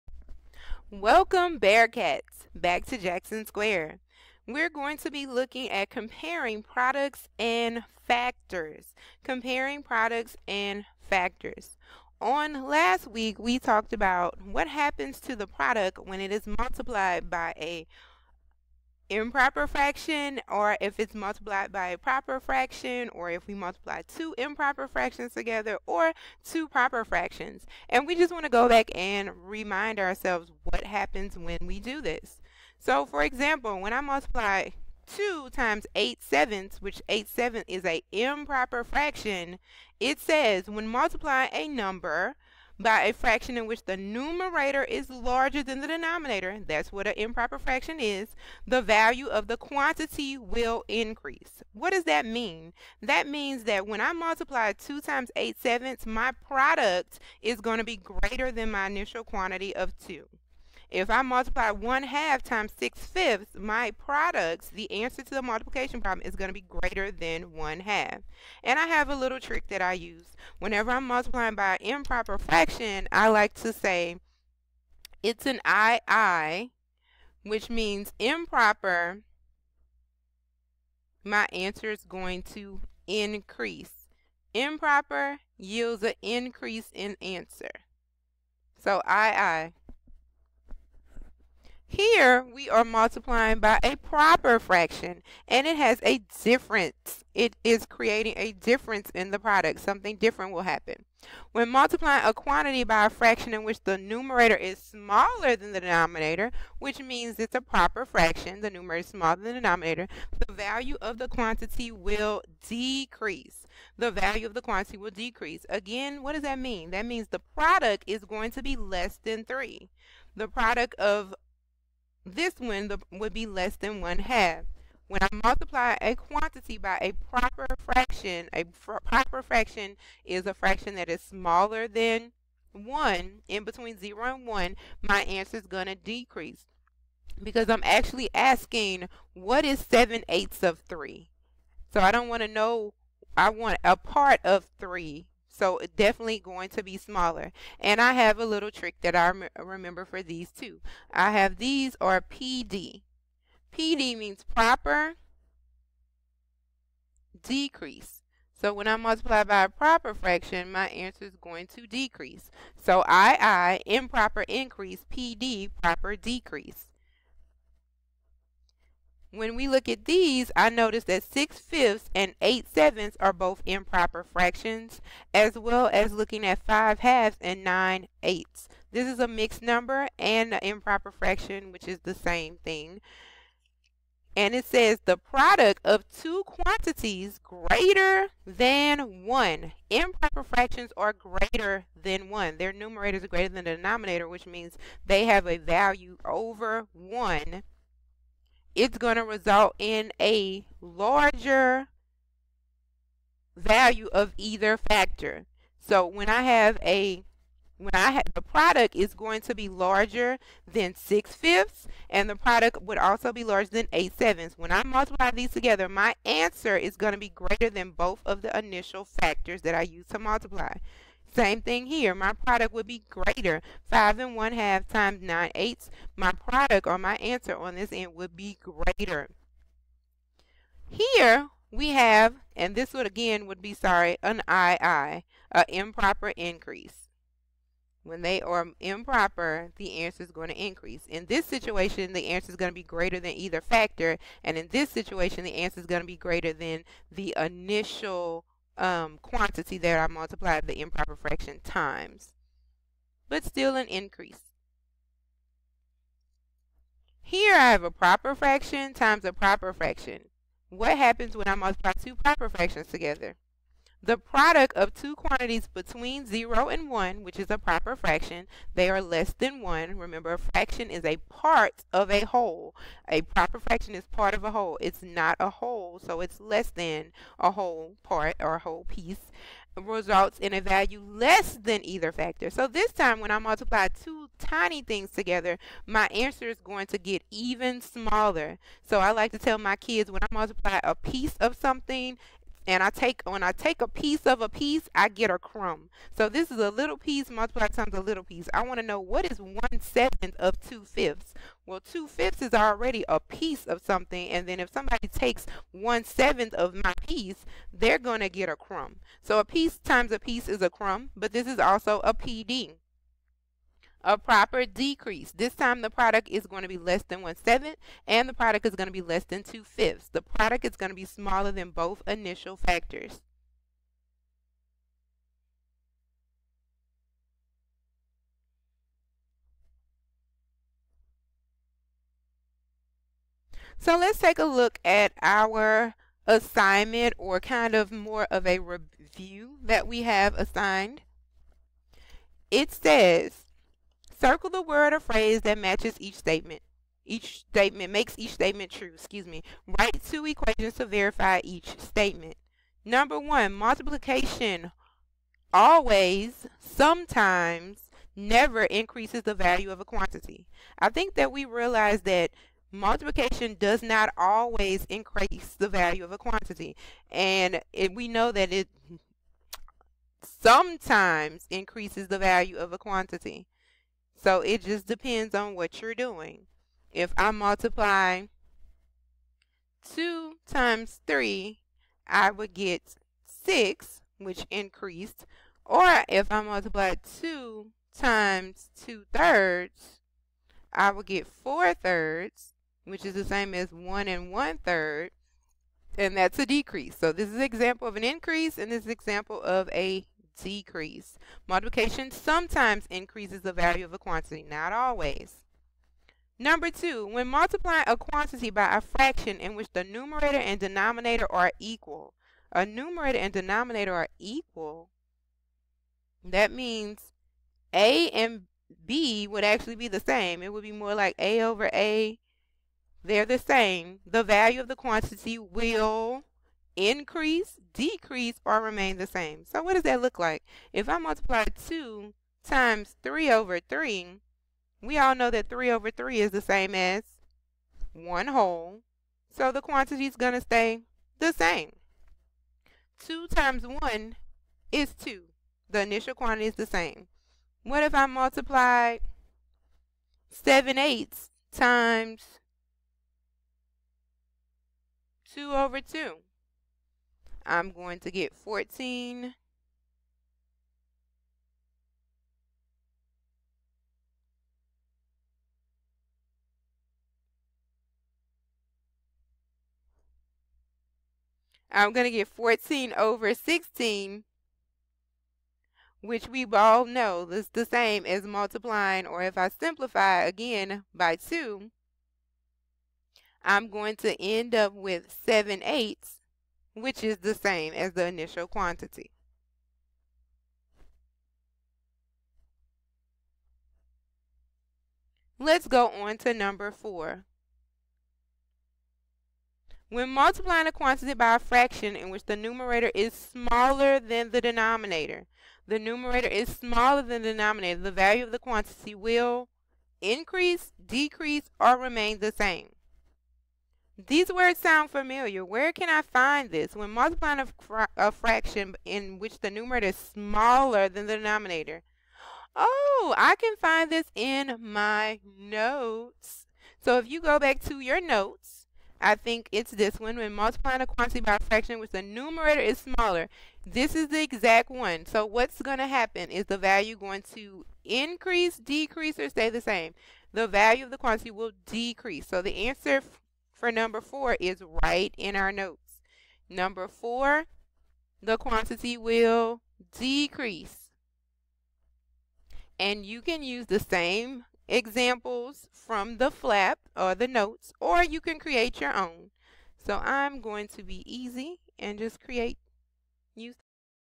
Comparing products and factors when multiplying fractions
What happens to a quantity when multiplied by a proper or improper fraction?